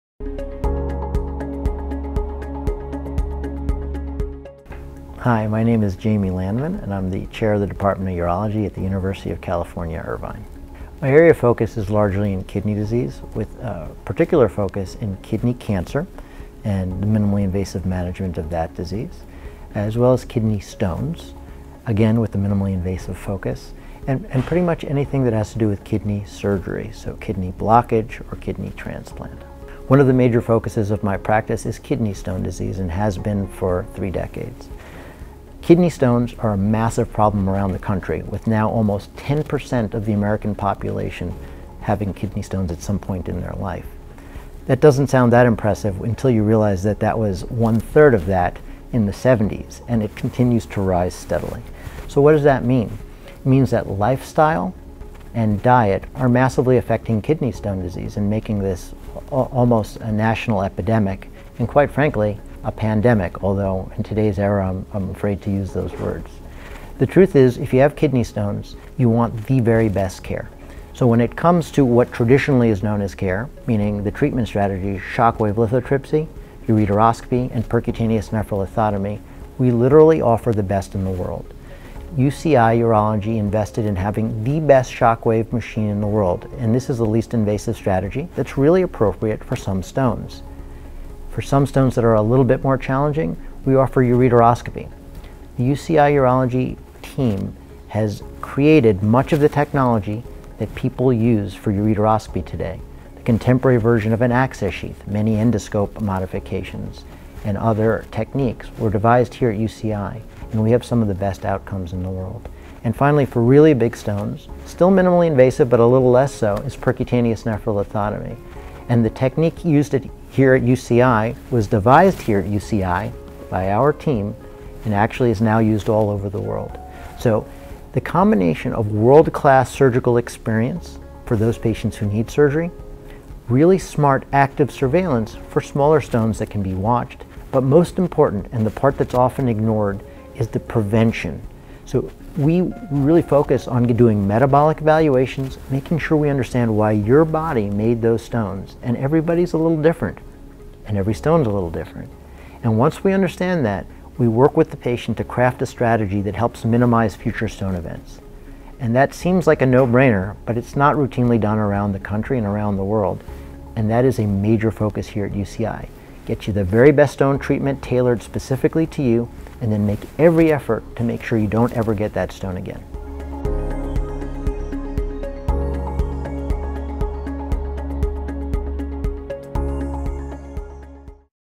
Kidney Stone Disease By Dr. Jaime Landman - UC Irvine Department of Urology
Dr. Landman is the chair of the Department of Urology at UC Irvine in Orange County, CA. He possesses extensive clinical expertise in endoscopic and laparoscopic kidney surgery, and his research has provided patients with more precise treatment. He is also a kidney stone specialist and expert in BPH treatment.

Dr. Landman is considered an international expert in the management of kidney cancer and is a pioneer on the application of minimally invasive techniques for ablation of kidney cancer. He has helped to re-define the diagnosis of kidney cancer through the development of novel diagnostic biopsy techniques in his work as a urologist.